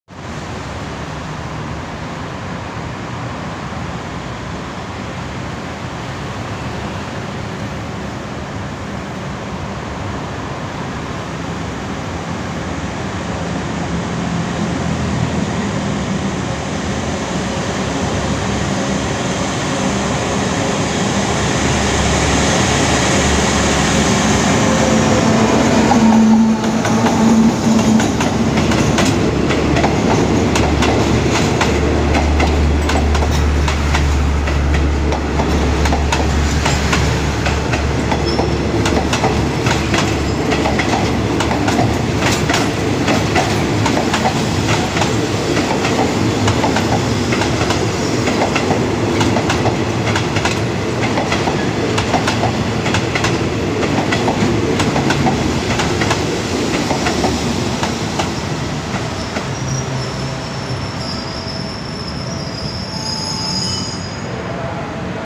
ขบวน 140 ALS.4133 ถึงหลักสี่
ALS.4133 ทำขบวน 140 ถึงหลักสี่ เวลา 6.47 น. ช้า 16 นาที
5 ก.ย.65